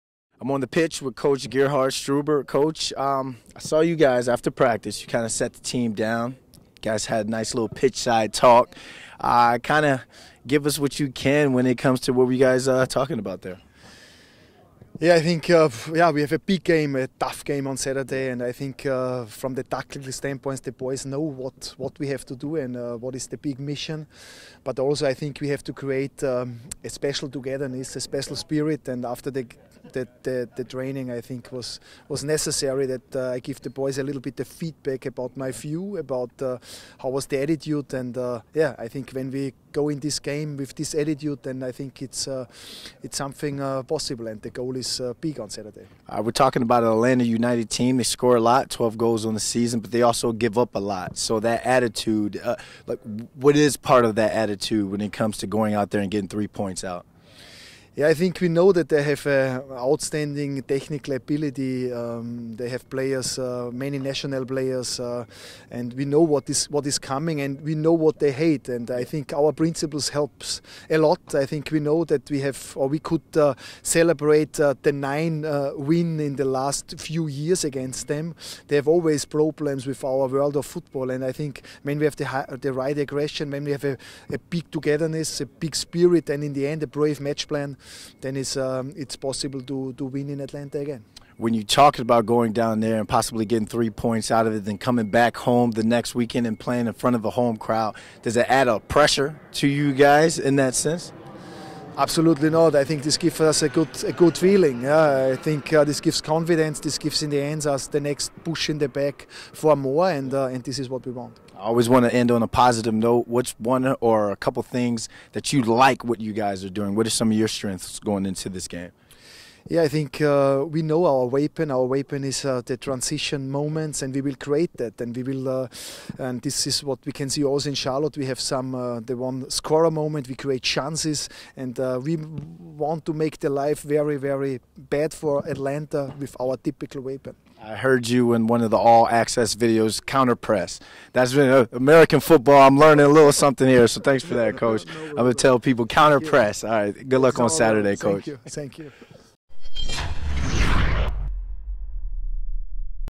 NY Red Bulls vs Atlanta United preview: Coach Gerhard Struber gets candid | SNY
New York Red Bulls head coach Gerhard Struber joins Brandon London to preview their matchup vs Atlanta United this weekend.